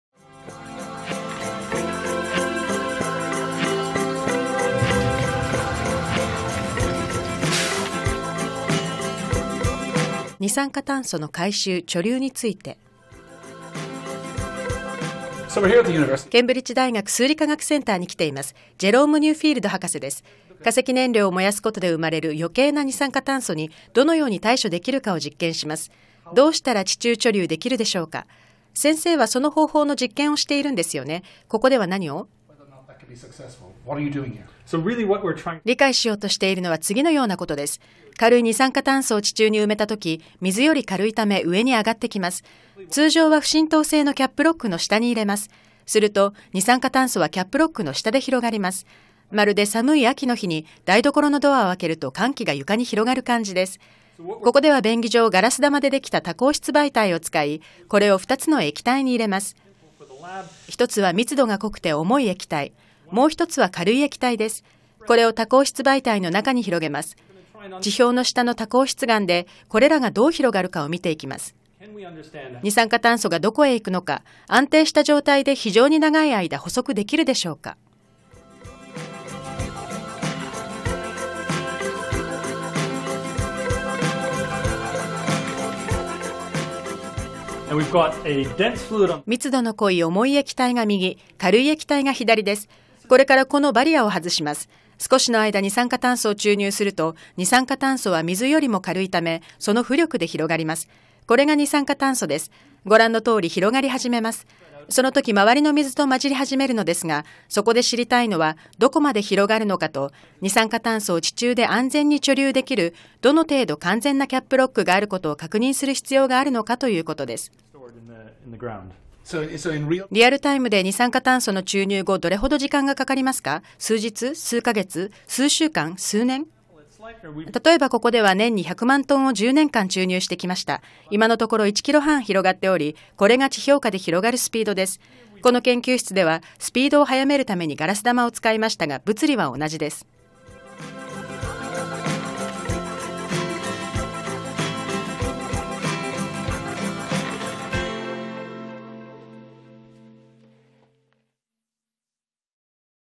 温暖化対策の有望技術「CO2貯留」 専門家に聞く
パリで現在開催中の気候変動問題を話し合う国際会議「COP21」では、温暖化ガスの排出を削減し気温上昇を抑制する取り決めで合意を目指している。一方、温暖化ガスの二酸化炭素を回収し、地中に閉じ込める研究も進められている。英ケンブリッジ大学の専門家に模型を使ってその仕組みを説明してもらった。